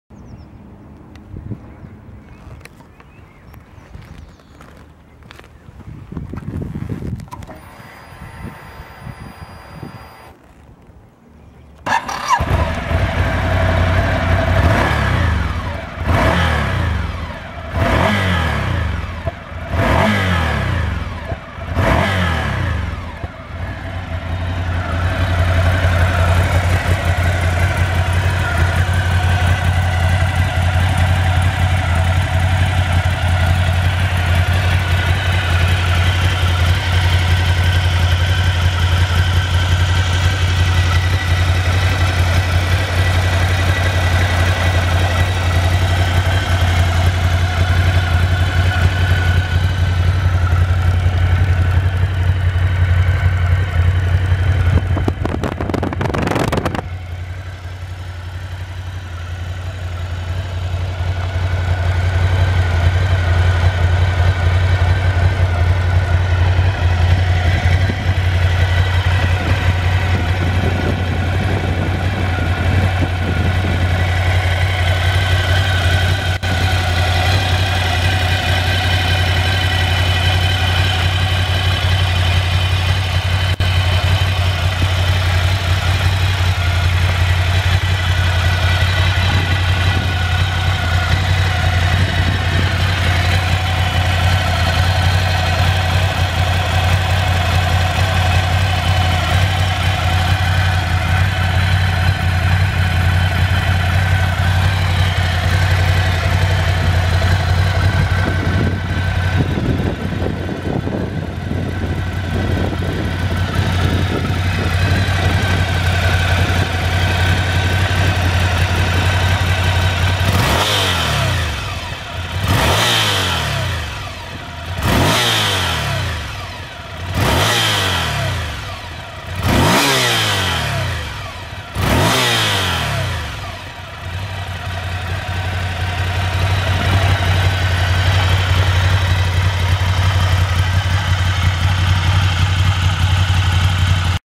Ducati Monster 797 Start up and Sound
Short video of the new Ducati Monster 797. Many thanks to Ducati Roma for this test ride.